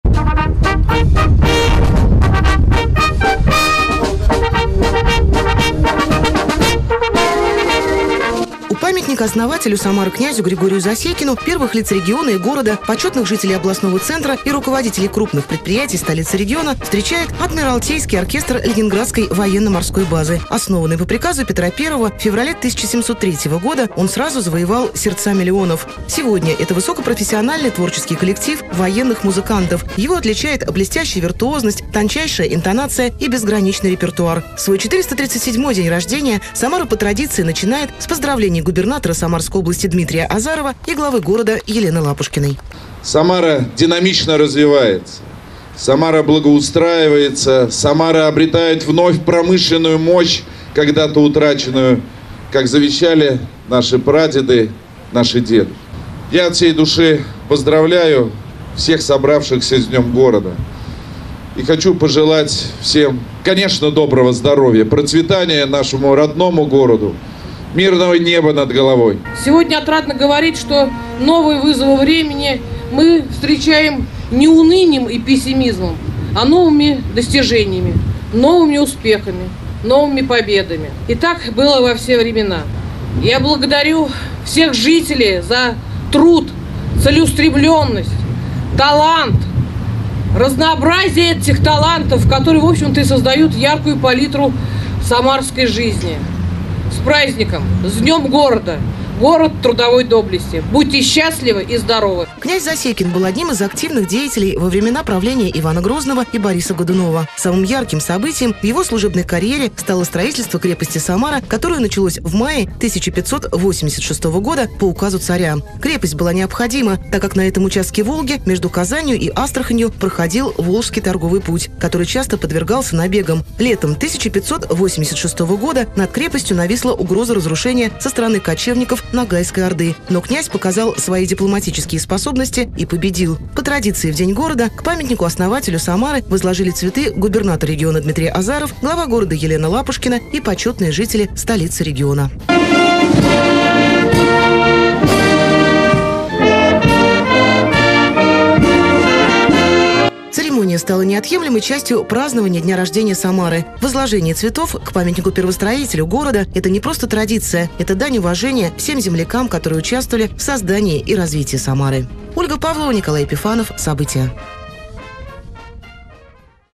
В Самаре в честь Дня города возложили цветы к памятнику Григорию Засекину
Цветы к памятнику основателю Самары князю Григорию Засекину возложили губернатор региона Дмитрий Азаров, глава города Елена Лапушкина и почётные жители столицы региона. Церемония​ стала неотъемлемой частью празднования дня рождения Самары. Для первых лиц региона и города, жителей и гостей областного центра выступили духовые оркестры нашей страны. В том числе Адмиралтейский оркестр Ленинградской военно-морской базы. Подробности в репортаже Ольги Павловой.